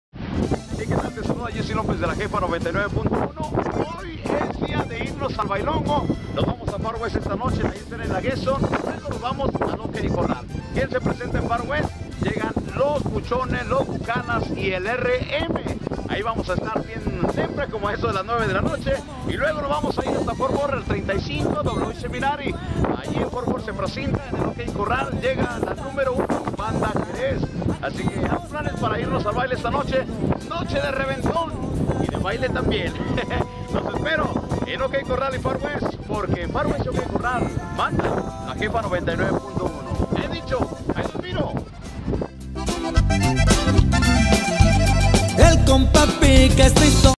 Farwest Dallas Y Ok Corral Fort Worth
Esta Noche Los Buchones / Los Bukanas Y El RM Ademas Banda Jerez Checate El Video Para Mas Informasion..